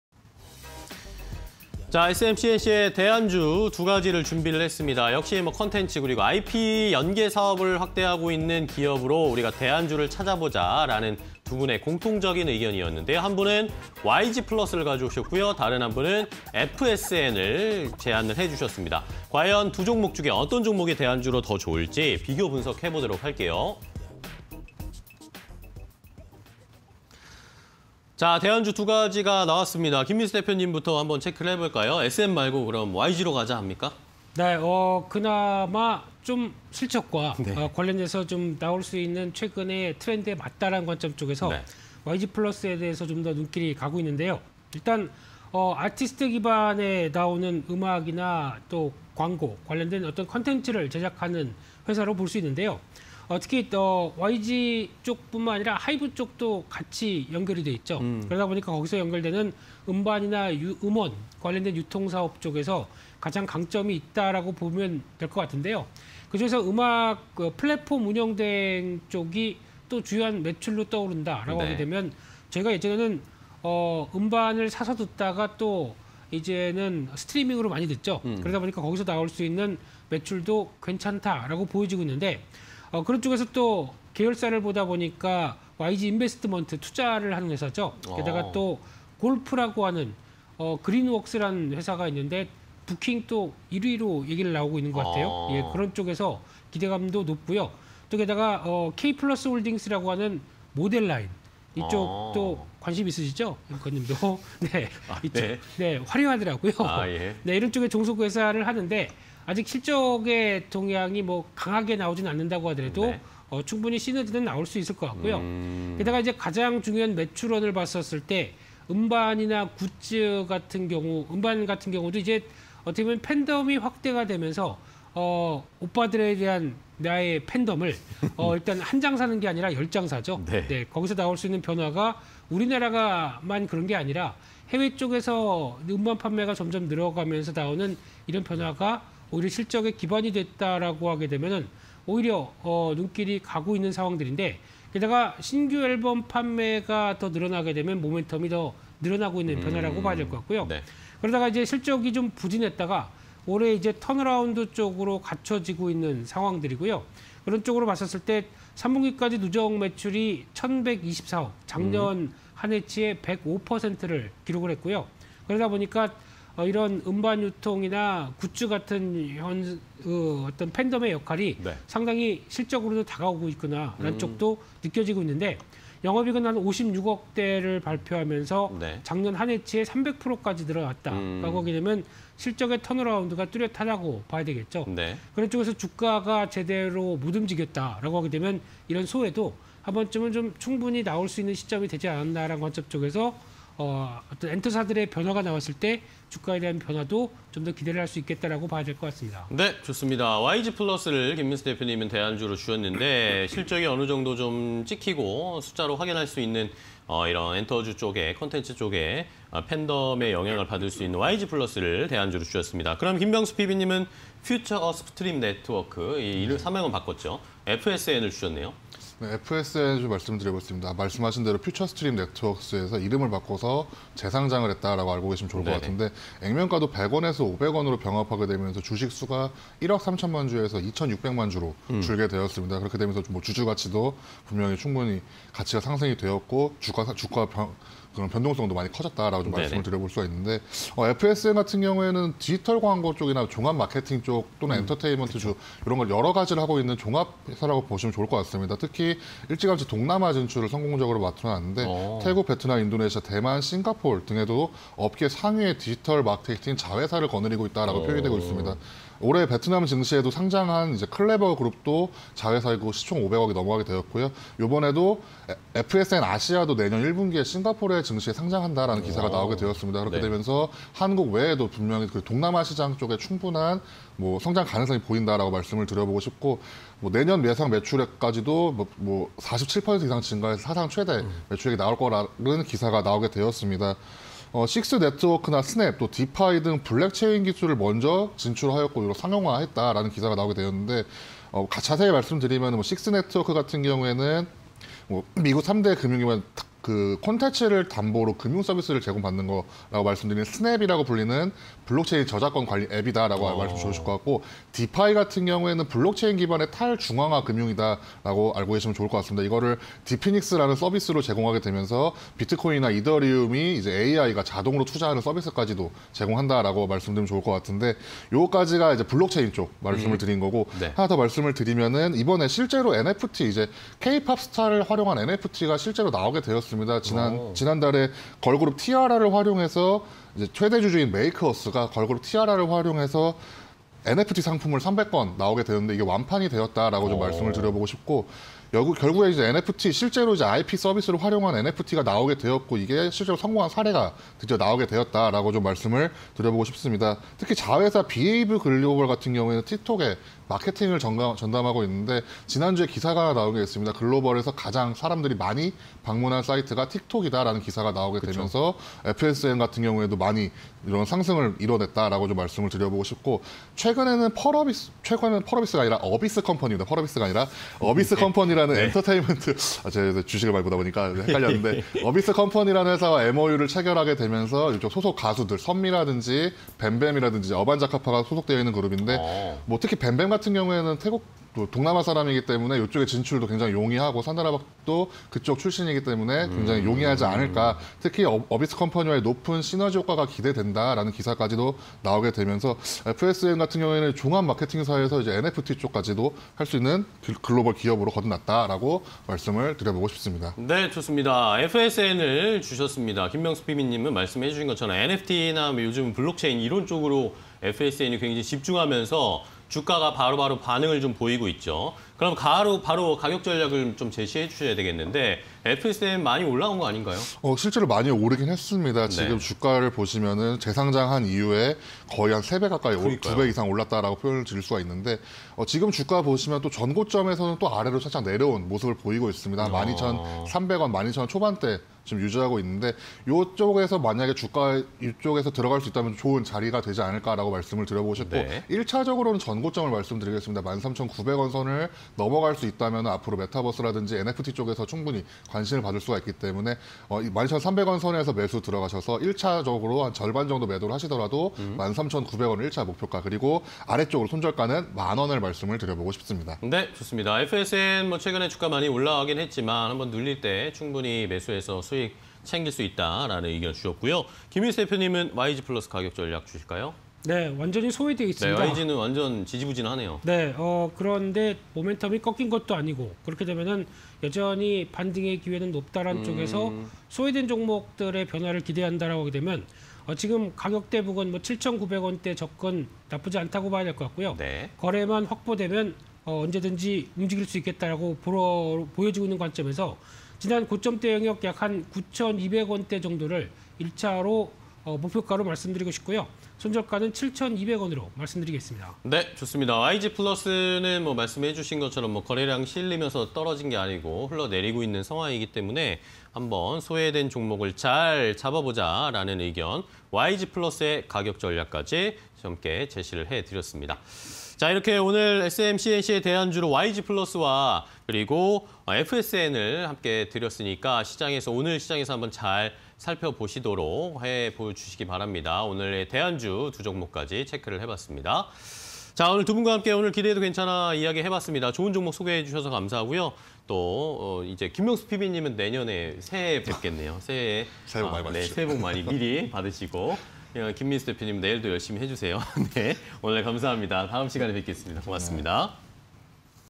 YG PLUS VS FSN... IP 연계사업 확대_대안주를 찾아라 (20211227)
- IP 기반 음악, MD 등 콘텐츠 제작
- 다양한 분야의 종속회사 보유
- 팬덤 강화… 굿즈 판매 확대 기대
- 올해 실적 턴어라운드 전망

- 종합 마케팅 사에서 NFT 선두로
- 퓨처스트림네트웍스→FSN으로 재상장
- 동남아 진출 및 안착… 자회사, 해외 상장
- 블록체인 기반 NFT 기술 상용화 先진행
- 어비스컴퍼니 MOU NFT 시장 다각화
- 소속 아티스트 IP로 동남아 공략 기대